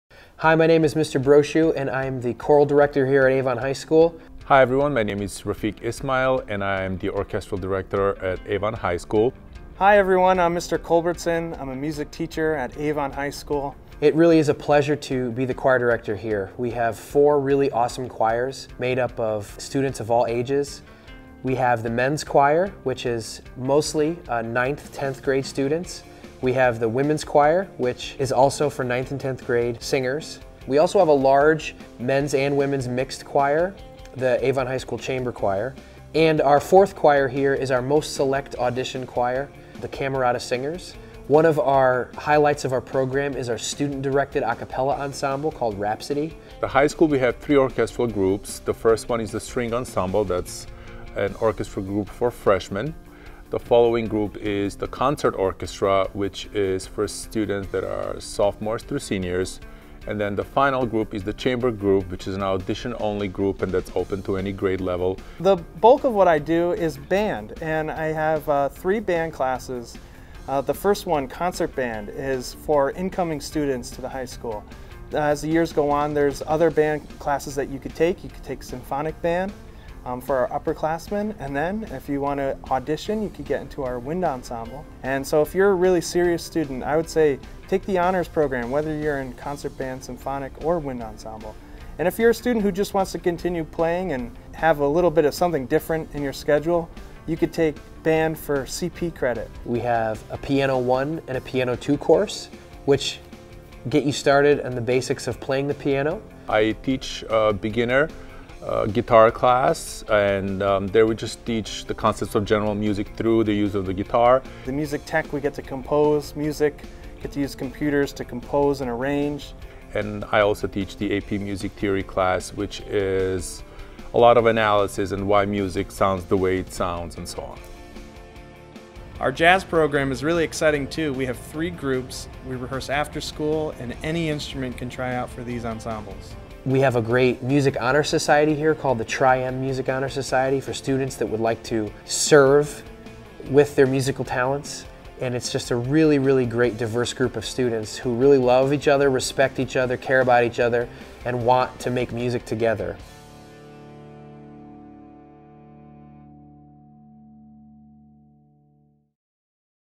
Avon High School (CT) Music Department Tour
Take a video tour of the music department at Connecticut's Avon High School, meet its directors, and learn about all of the opportunities that the department has to offer!